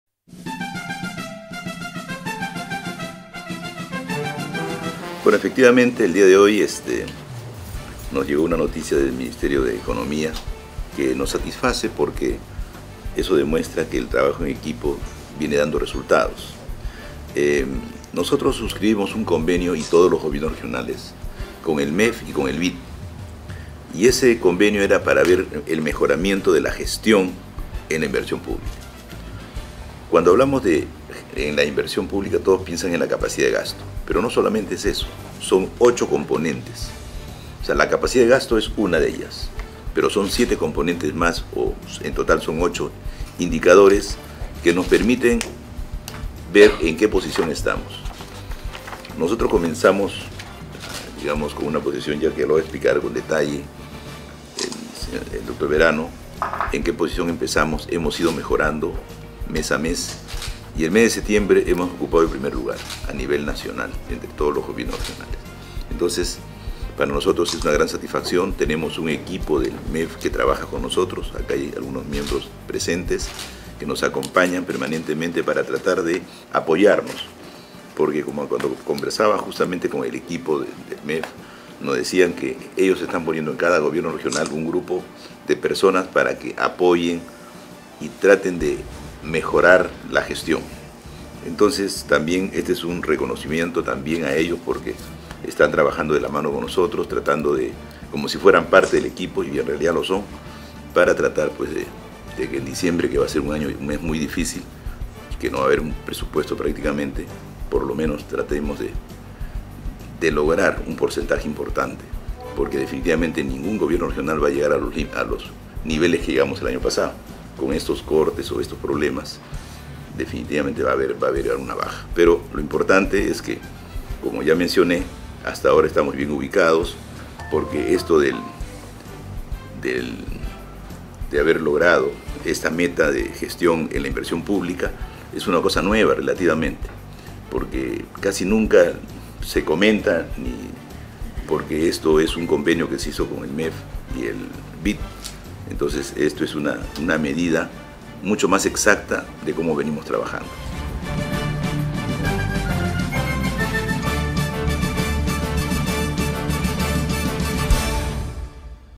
Gobernador Regional Ing. Nelson Chui Mejía dio a conocer los logros de gestión alcanzados a la fecha
En conferencia de prensa, el gobernador regional Ing. Nelson Chui Mejía dio a conocer los logros de gestión alcanzados a la fecha.
Explicó del trabajo responsable y planificado que ha permitido al Gobierno Regional de Lima ocupar el primer lugar del Ranking de Gastos de Inversiones a nivel nacional y que redundará en el beneficio social y económico real para la población de las nueve provincias.